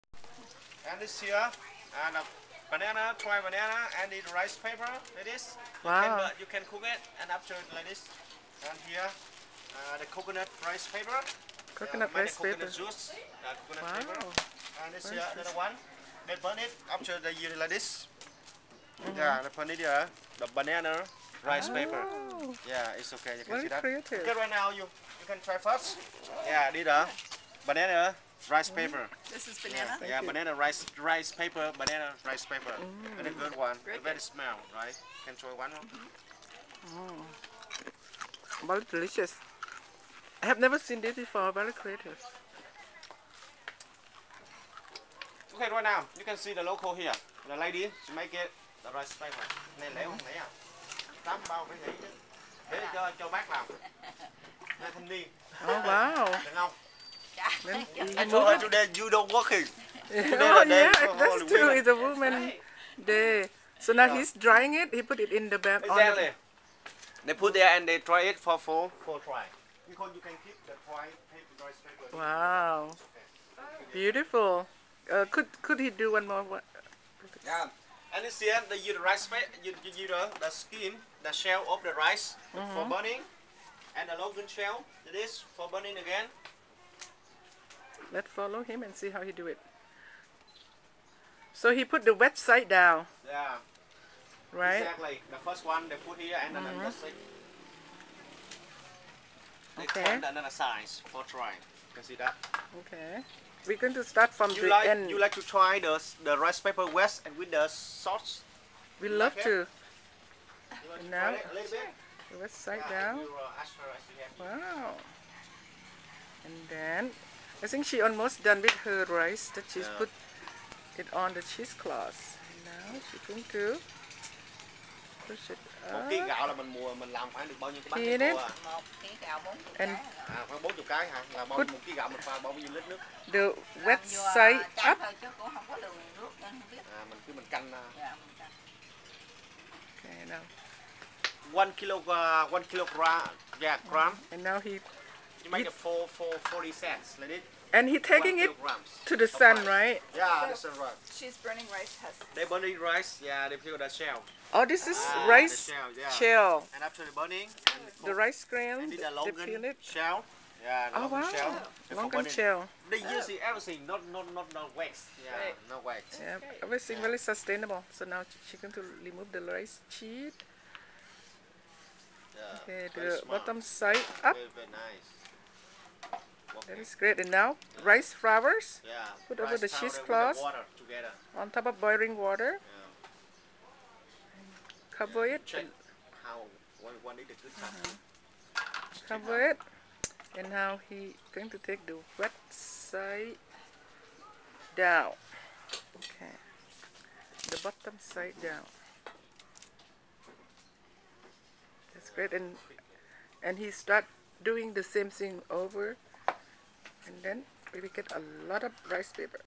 Vietnamese Rice Paper
Pranee Halvorsen from I Love Thai Cooking and tour leader Mr. Fu lead a tour to Mekong Delta to see the rice paper being made.